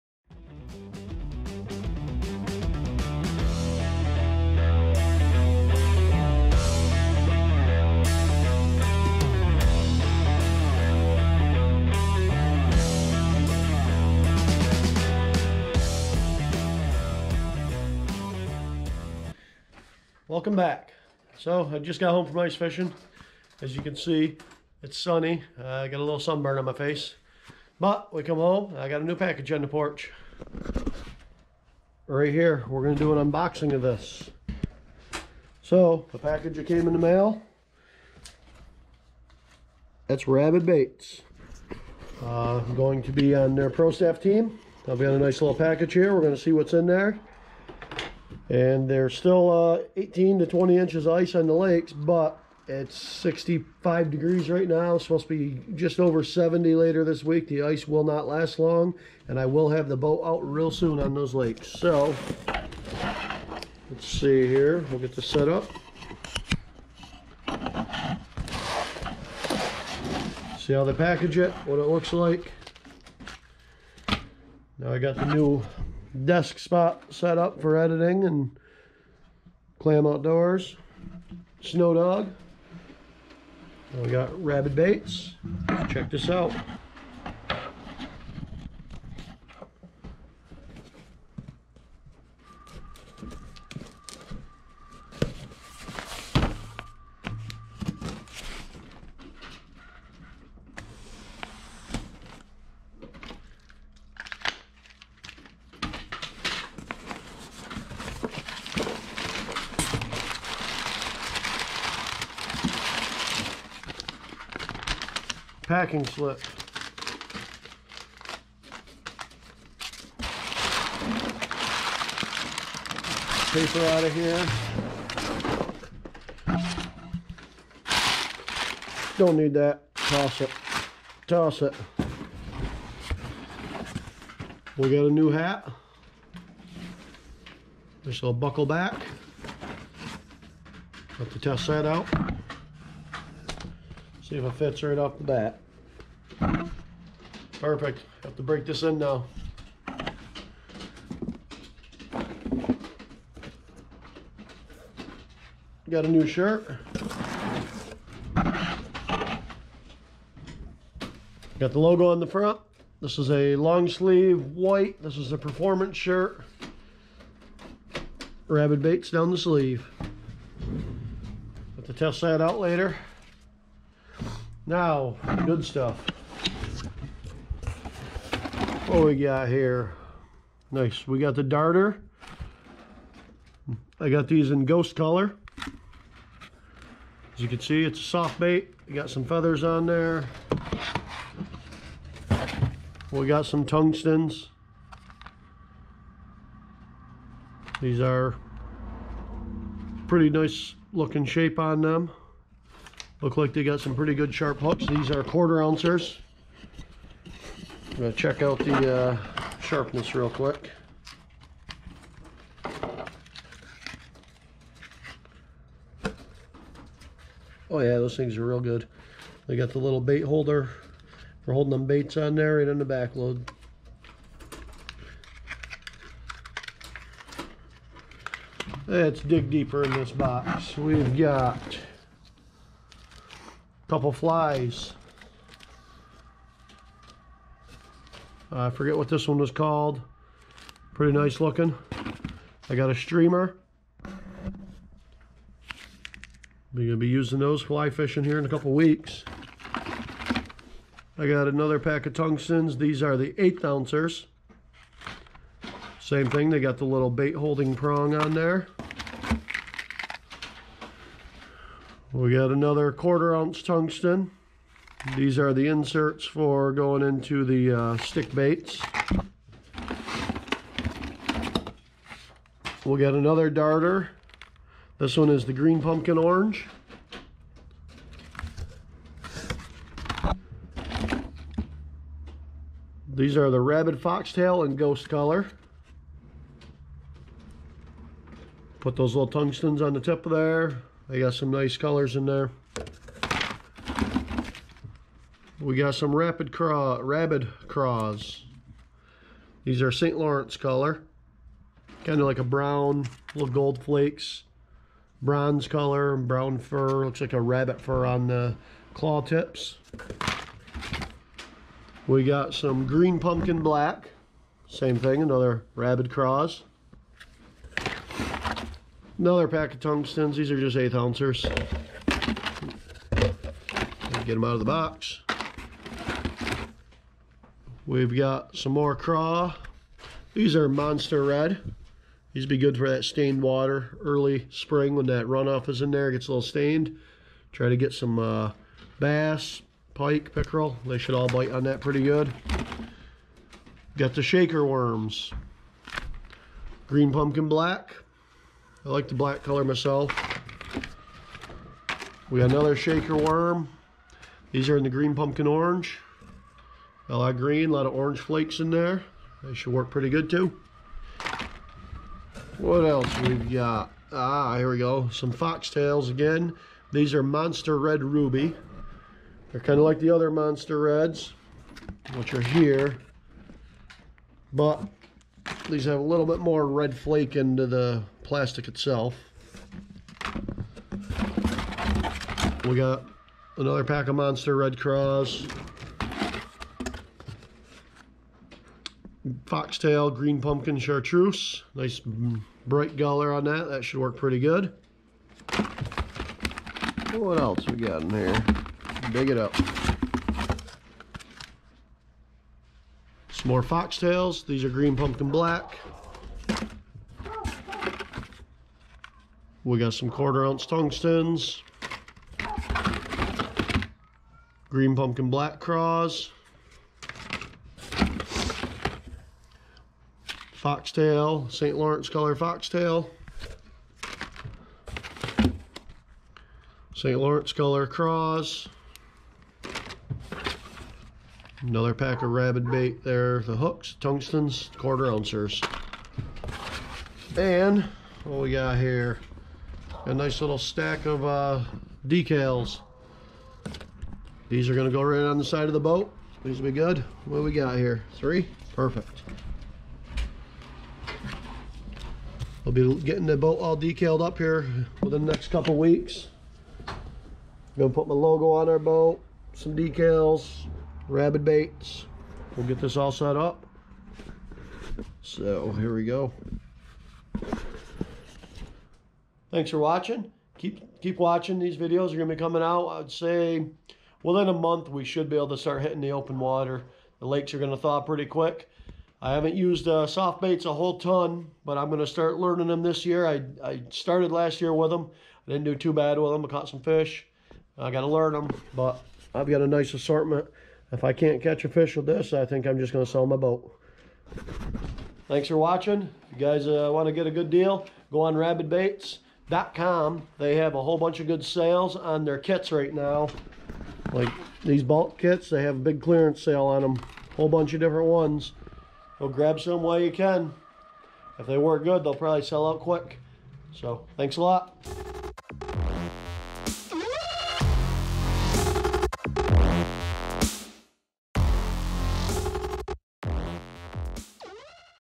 I got my new RABID BAITS !! Will Also be working with them on their Pro Staff Team! Unboxing Video!
I will be working this fishing season with Rabid Baits soft baits for bass and pike fishing!! I have the boat ready and new tires on the Trailer! Ill be hitting the open water very soon testing these baits out!! Ill have camera along for my first use with them and see How they hang!